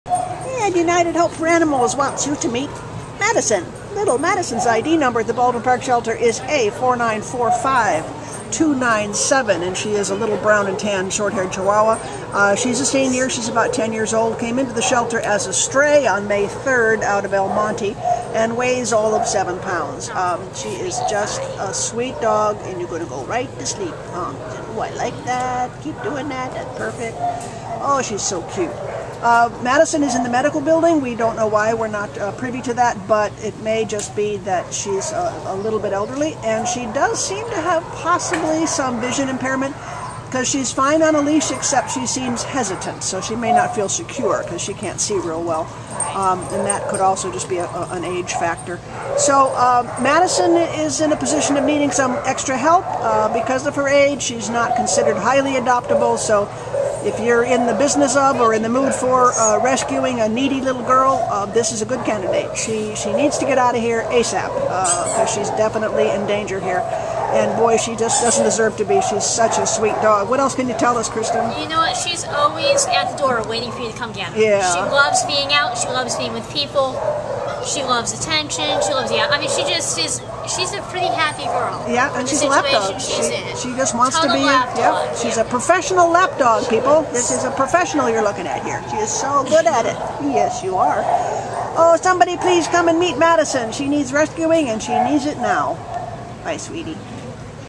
A4945297 Madison
Madison is a lovely 10-year-old brown and tan female Chihuahua who was found in El Monte and brought to the Baldwin Park Animal Care Center on May 3rd. Weighing just under 7 lbs, Madison walks a little hesitantly on a leash, but is easy to manage. She loves people and enjoys going outside to explore. She has shown signs of being housebroken and seems to get along okay with other dogs. Madison will thrive as an indoor companion with just about anyone in any living situation, especially a senior or anyone living in a quiet household.